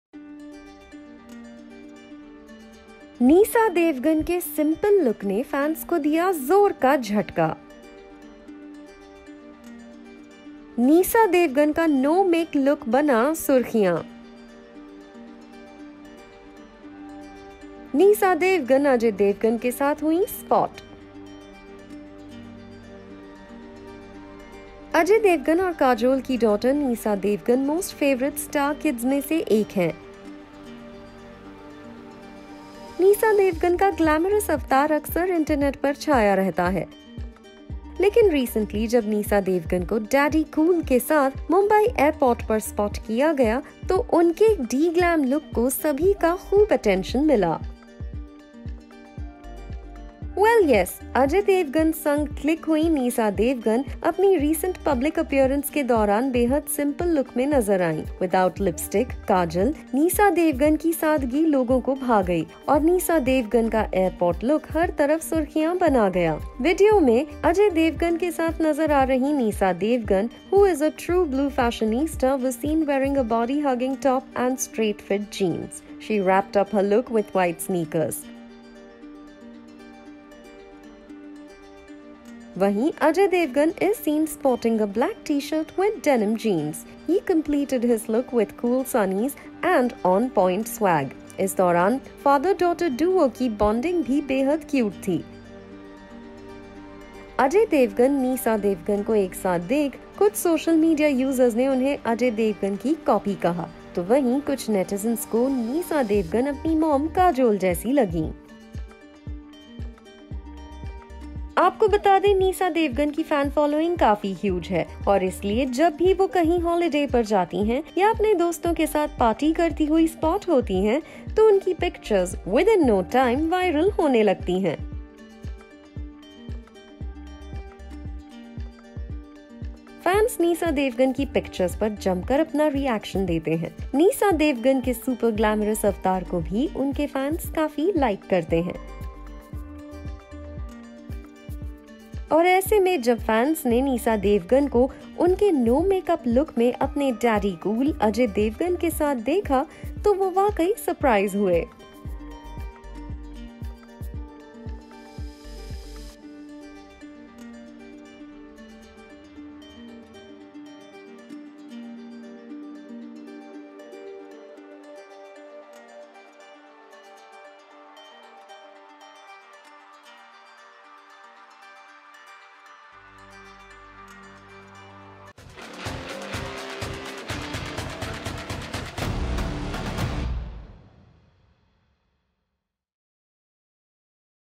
जब एयरपोर्ट पर एक साथ ठहाके लगाते दिखे Ajay Devgan-Nysa Devgan| Bollywood News
बेटी नीसा के साथ एयरपोर्ट पर अजय देवगन हुए स्पॉट. बेटी के साथ हंसते-हंसते बात करते नजर आए अजय देवगन. बेटी के साथ अजय को इतने कूल अवतार में देख लोग कह रहे रिश्ता हो तो ऐसा.